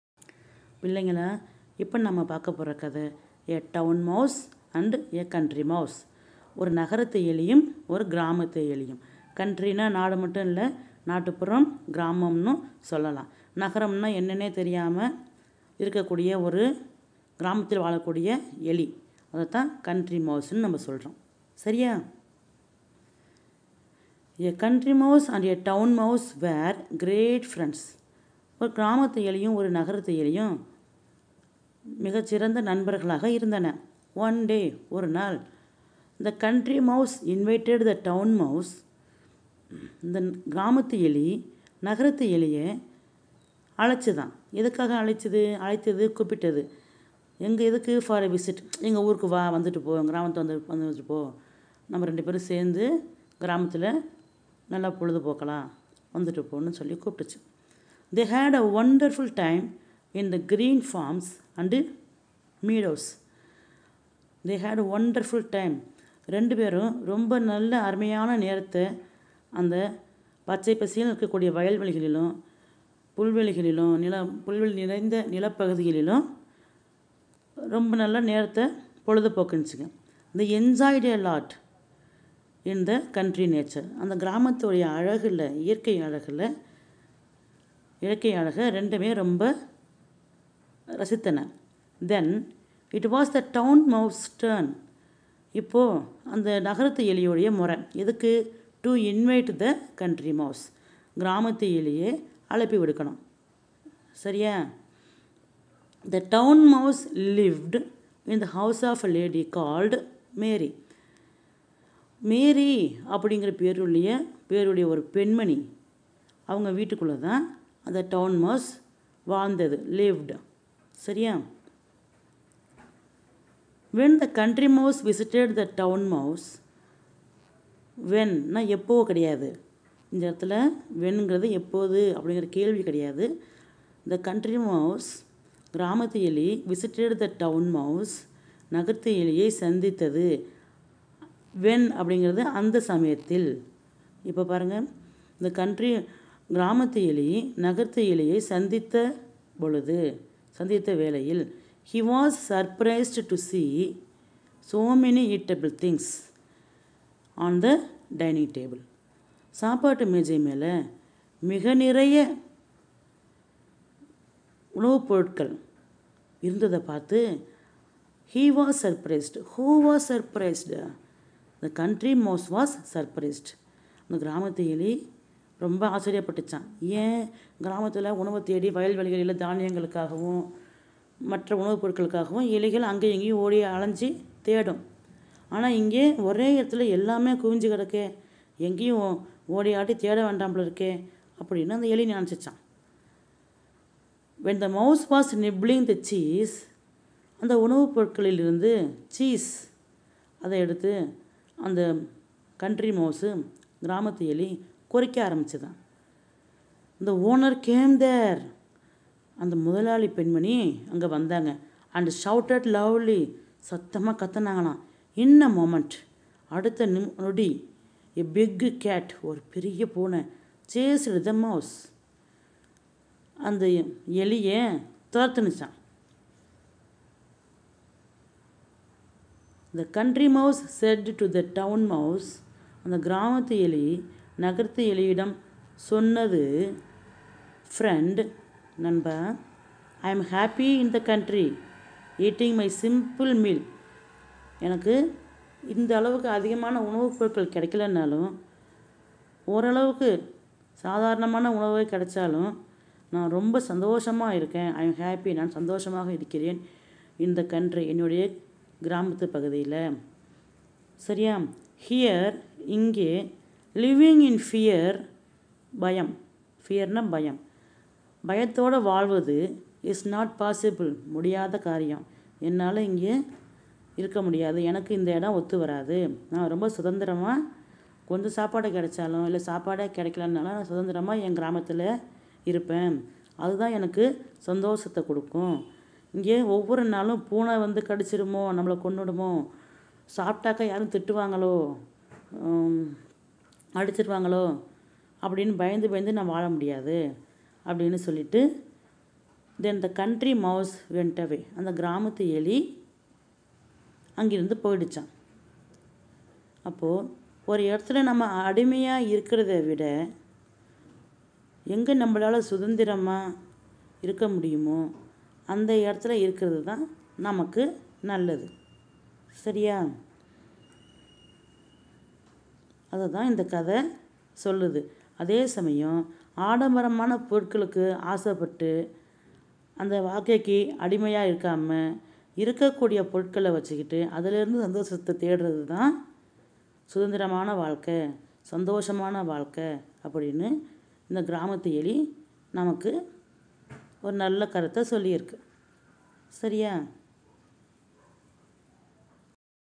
A TOWN MOUSE AND A COUNTRY MOUSE | ஒரு நகரத்து எலியும் ஒரு கிராமத்து எலியும் (நீதி சிறுகதை)
A short moral story which teaches to live happily with what you have and that too with freedom even though you
are not so rich.